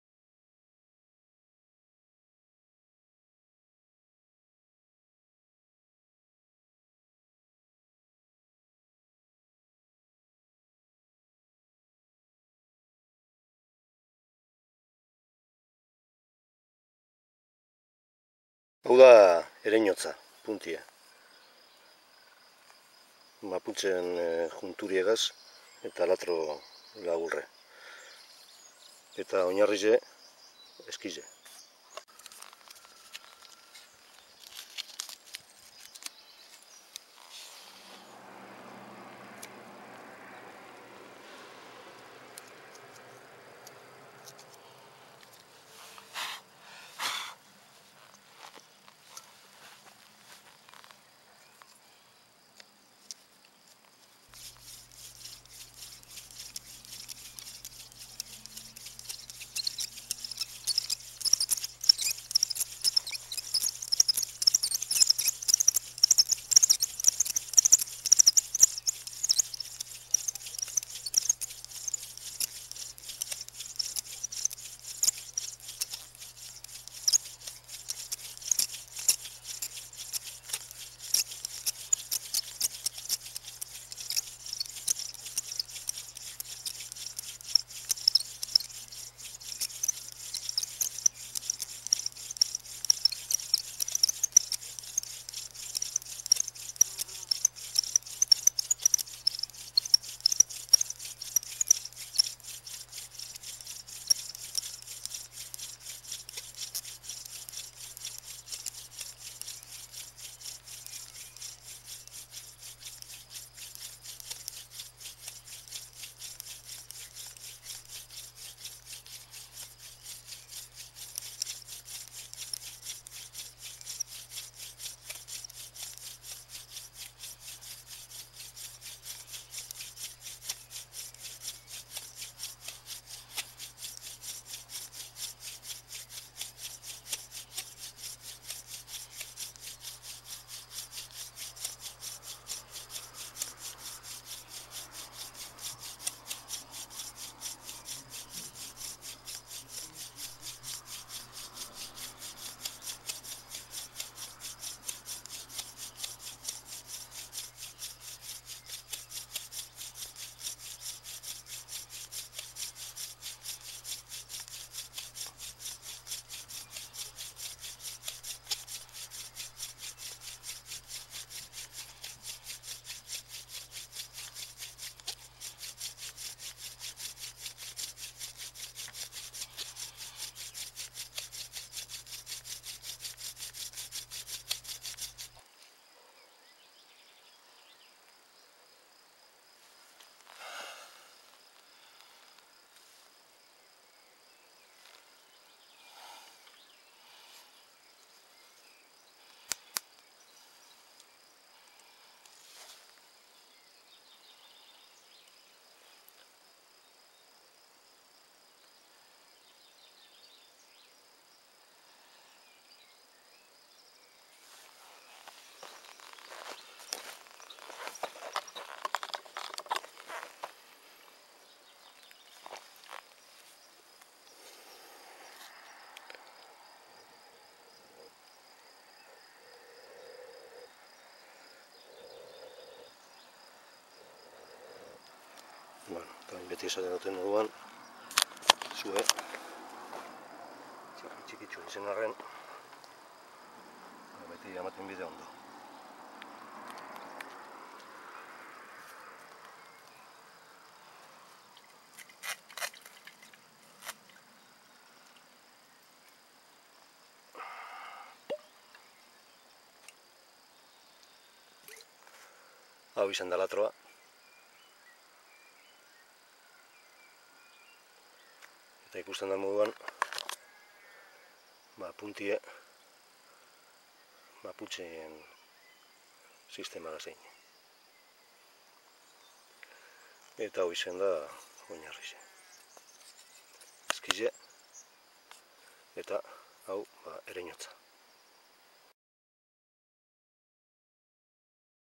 Mapuche joint, floating hands system / Mapuche juntura/Empalme Mapuche manos flotantes
Continuing with the floating hands system (1), which I presented in the previous video this time I present a video in which I apply the Mapuche joint (2) to the mentioned system.
The objective was to verify the viability of such a seemingly fragile joint, using a system in which it’s very difficult to maintain the verticality of the spindle when we rotate it.

Continuando con el sistema de las manos flotantes (1), que presente en el vídeo anterior ), en esta ocasión presento un vídeo en el que aplico el empalme Mapuche (2) al mencionado sistema.
El objetivo era comprobar la viabilidad de un empalme tan aparentemente frágil, utilizando un sistema que presenta mucha dificultad para  mantener la verticalidad del taladro cuando lo hacemos girar.

Lehengo bideoan aurkeztu nuen sistemarekin jarraituz, esku flotagarriak (1), ), orain beste bideo bat aurkezten dut Mapuche juntura (2) aplikatuz
 Honen bezalako sitema hauskorraren erabilgarritasuna egiaztatzea zen nire asmoa. Sistema hauekin daratuluaren bertikaltasuna mantentzea, birarazten dogunean, oso zaila delako.